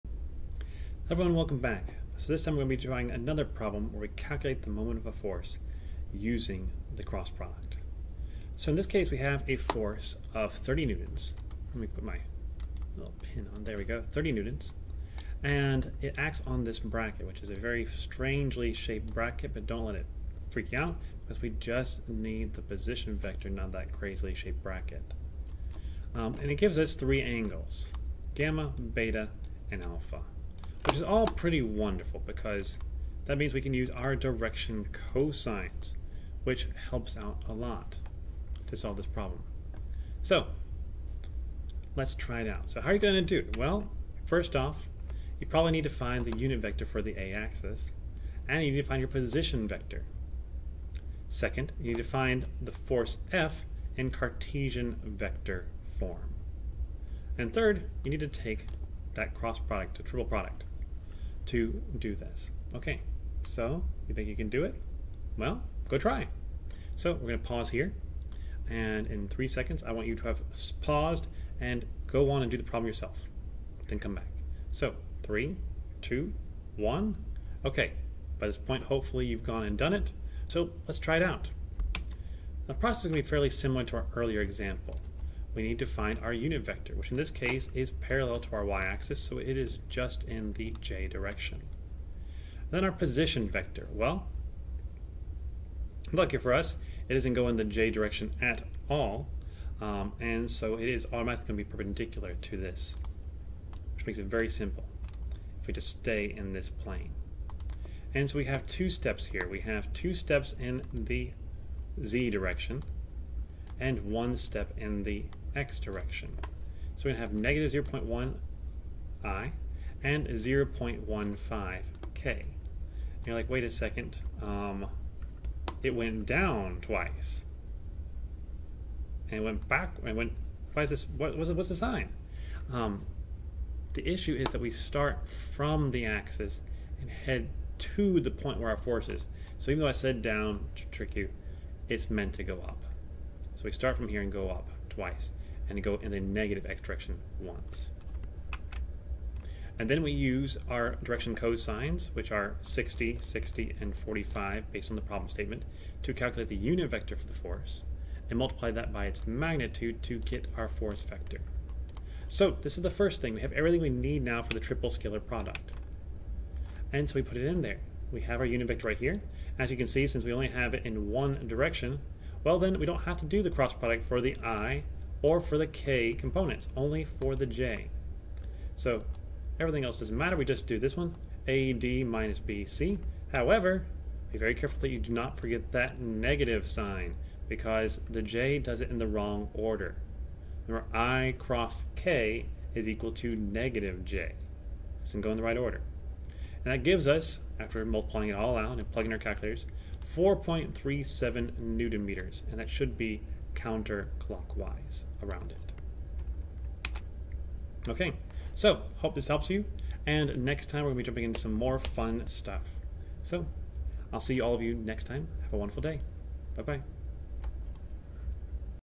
Group Problem: Triple Scalar Product
Triple scalar product sounds like some sort of up-and-coming band name. If you have a band named triple scalar product, please let me know and link your music in the comments. For now, though, this is a simple way to write out how you get the moment about a particular axis.